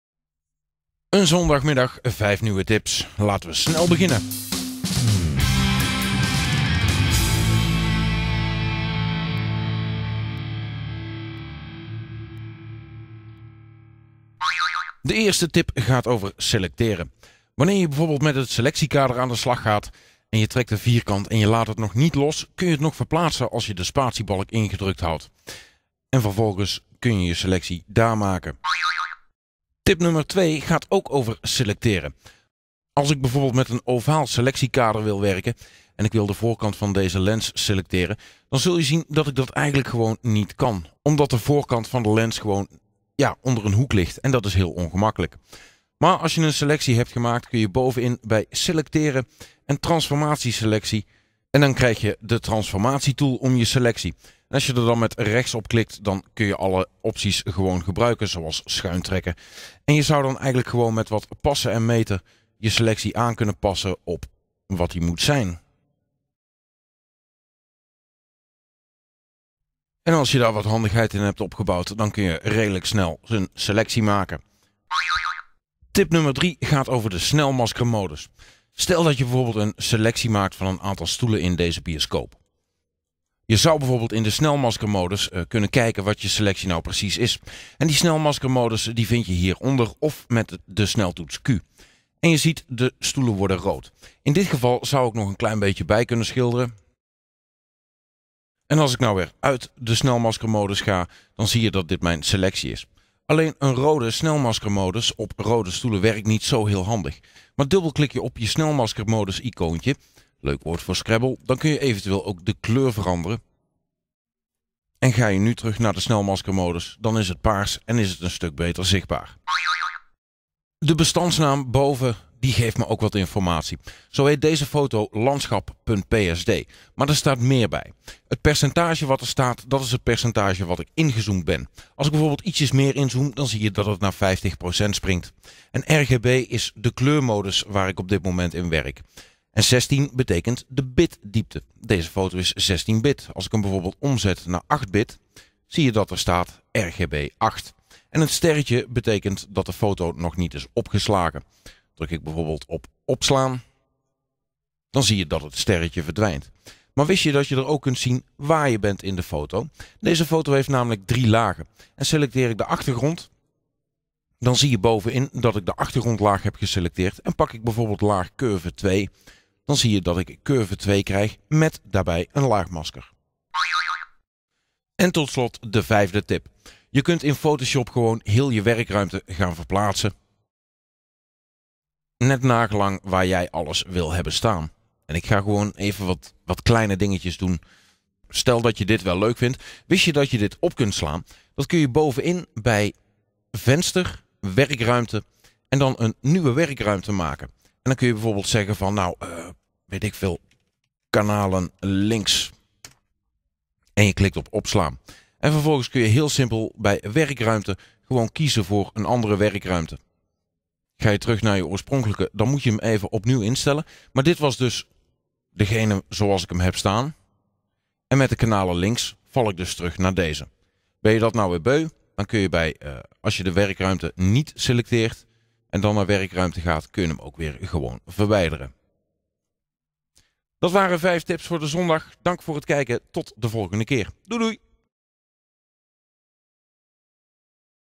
5 snelle tips deel III (Photoshop)
In deze aflevering van vijf tips over Photoshop:

- Het verplaatsen van een selectie voordat ie af is
- Het maken van een complexe ovale selectie
- Het veranderen van de kleur van je snelmaskermodus
- Uitleg over de naam van lagen
- Het opslaan van een andere werkruimte